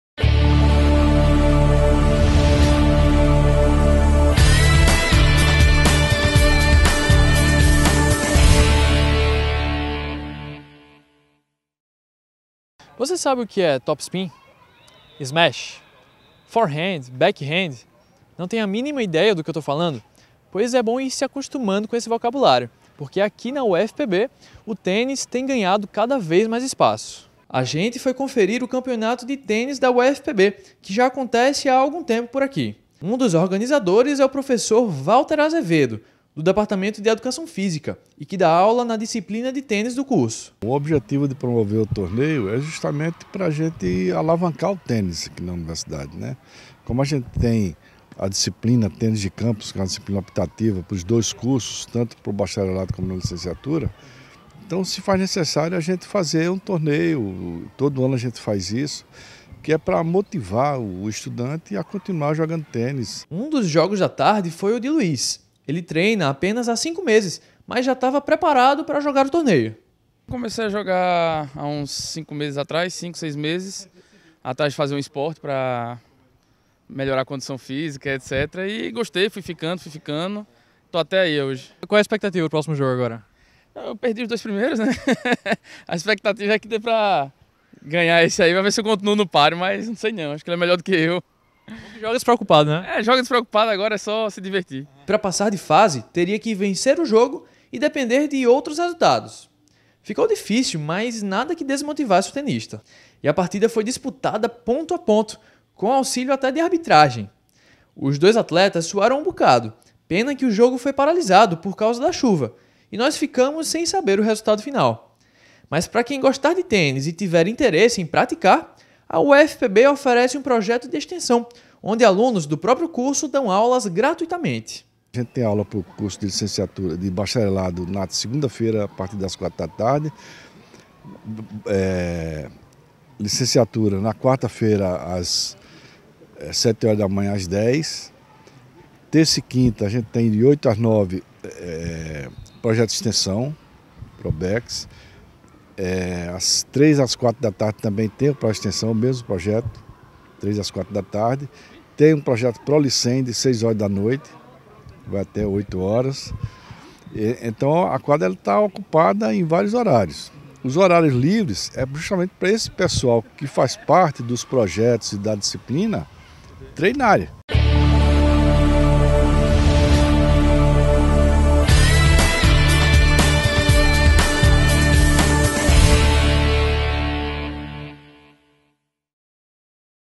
UFPB Acontece -Torneio de Tênis
Confira o torneio de tênis que aconteceu no centro de Educação Física da UFPB.